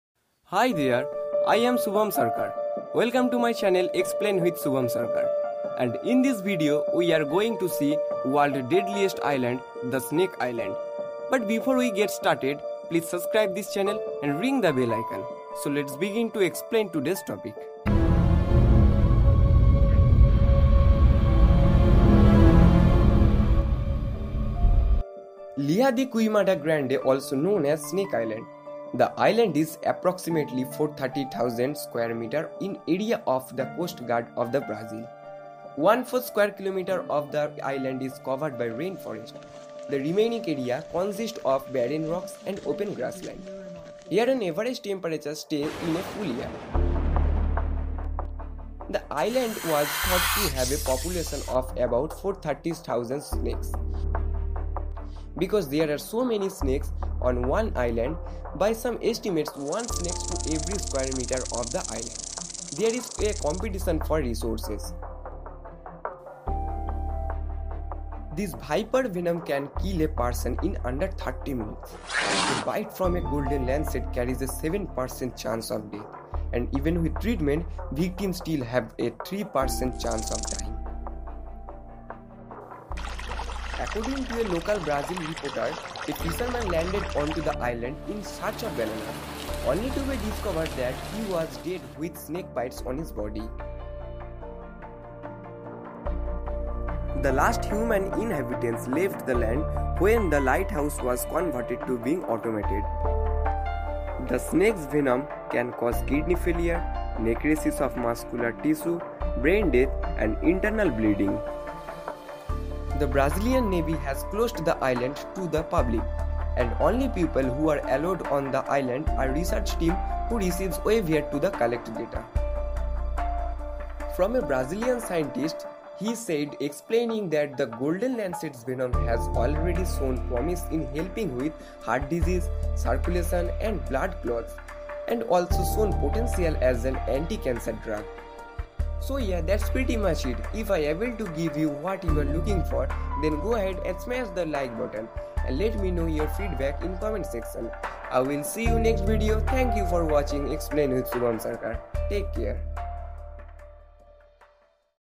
World Dangerous Island | Snake Island | Human Prohibited Place | Ilha de Queimada Grande | Brazil
Brazil’s Ilha de Queimada Grande is the only home of one of the world’s deadliest, and most endangered, snakes. These vipers' venom can kill a person in under an hour, and numerous local legends tell of the horrible fates that awaited those who wandered onto the shores of "Snake Island."

This Video is all about "Snake Island"

NB: "Explain With Subham Sarkar" is now "Bongo Onusondhan বঙ্গ অনুসন্ধান"

© Bongo Onusondhan বঙ্গ অনুসন্ধান